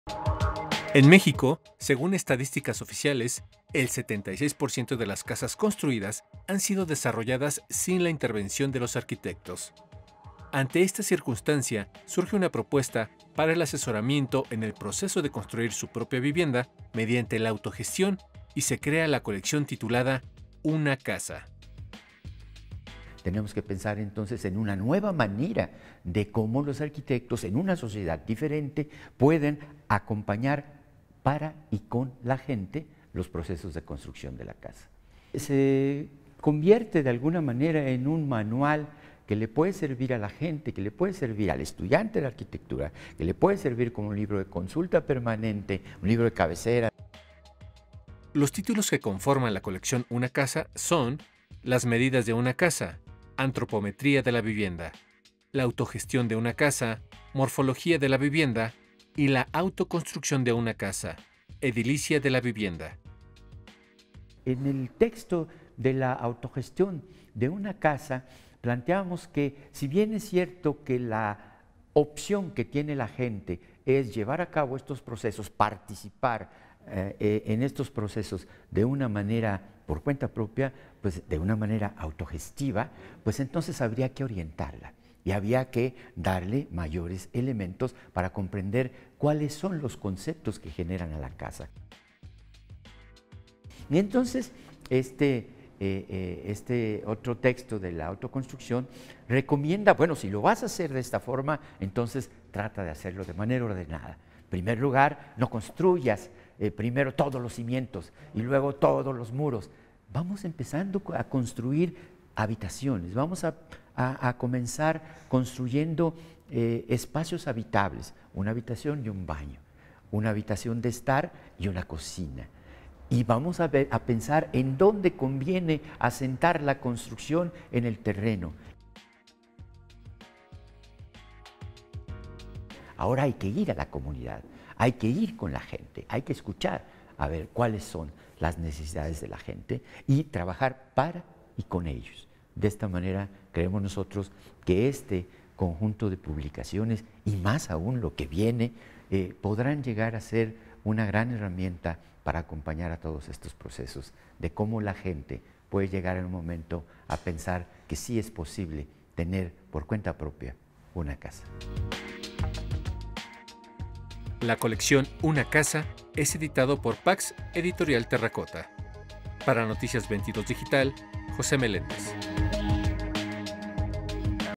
Colección "¡... una Casa!"
El 76 por ciento de las casas en México no tiene la participación de un arquitecto. Por ello la Colección ¡... una casa! busca ser una guía de construcción para quien decida construir su vivienda o incluso, como libro de cabecera para estudiantes de arquitectura.